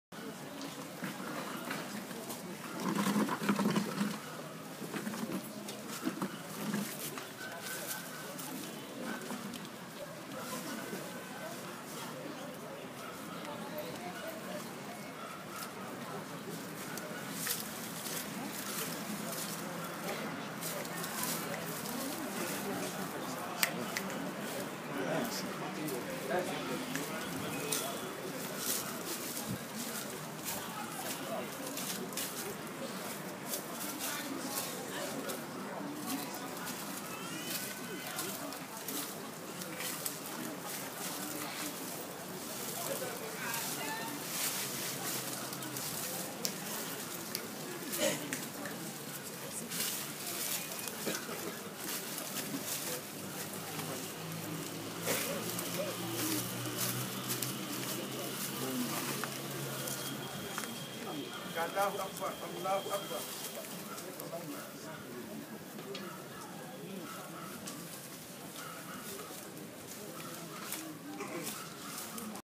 Cérémonie de lecture du Saint Coran au camp Boiro, le 25 ja
Cérémonie de lecture du Saint Coran au camp Boiro, le 25 janvier 2014.